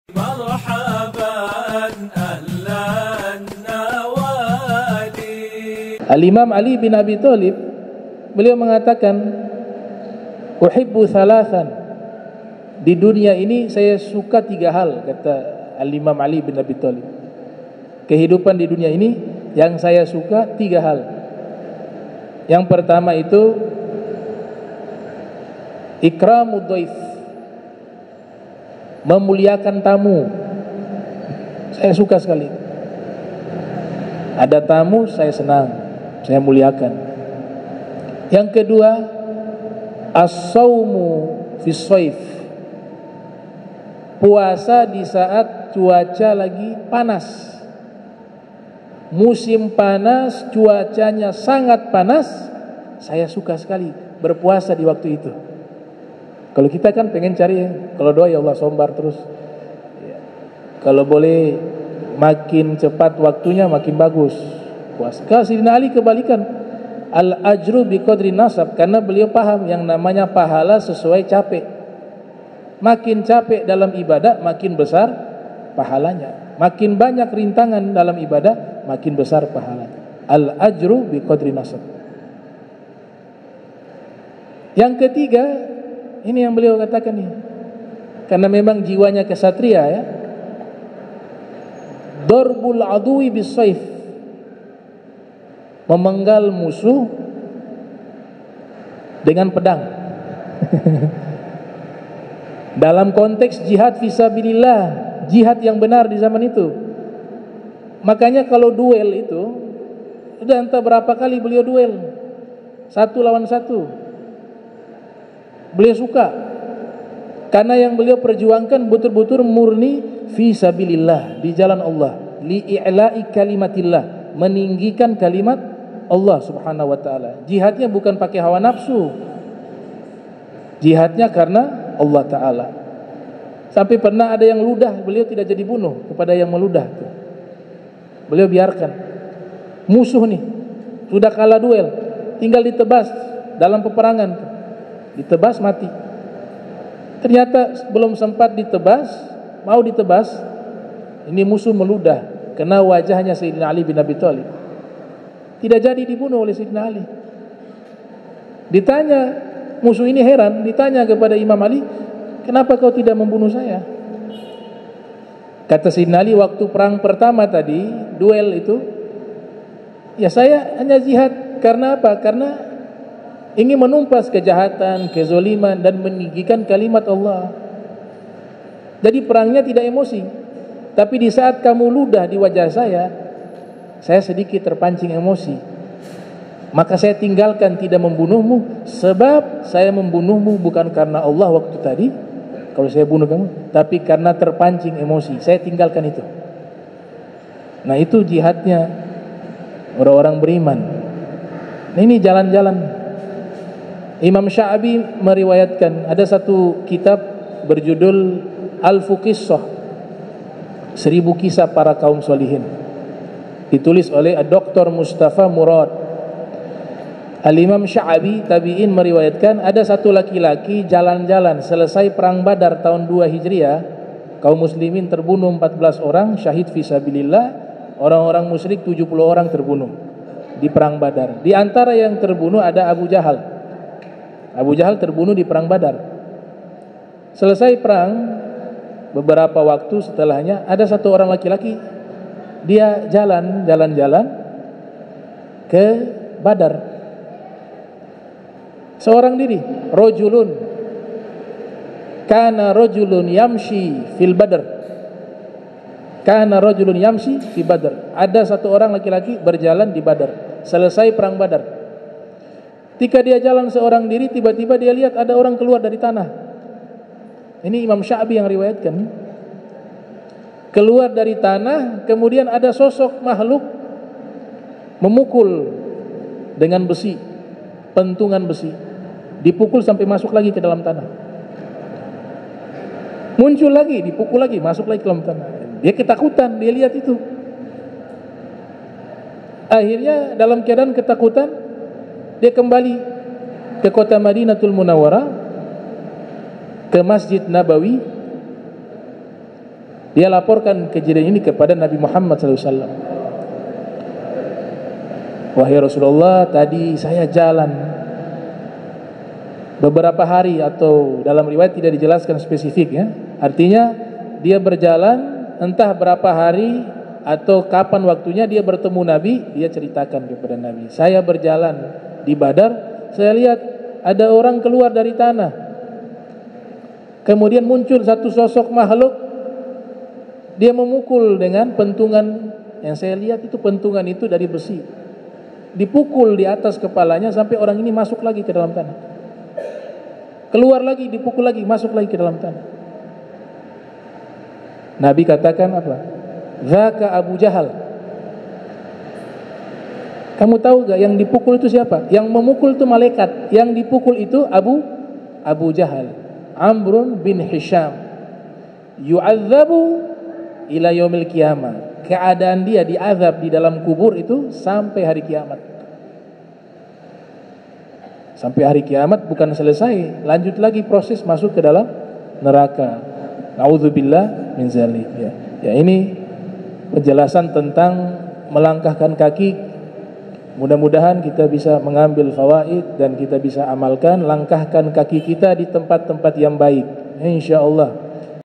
Kejadian Aneh Dipadang Badar | Habib Abdurrahman Assagaf
Kejadian Aneh Dipadang Badar
Kajian Ilmiah dari AlHabib Abdurrahman bin Kamel Assagaf MPdI  dengan menggunakan Referensi dari Kitab-Kitab Ulama Ahlu Sunnah Waljamaah yang telah diakui oleh mayoritas ummat Islam sejak Zaman Tabiin sampai sekarang.